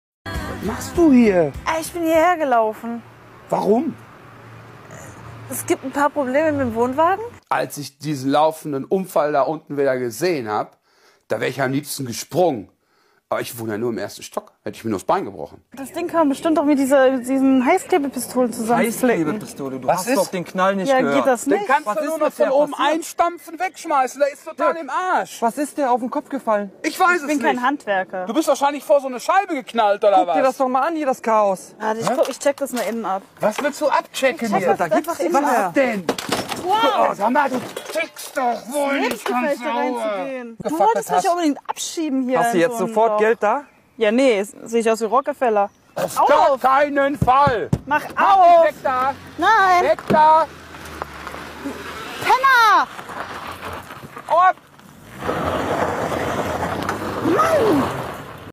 Dani der laufende Unfall
Dani macht ist ein einziger Unfall und macht alles kaputt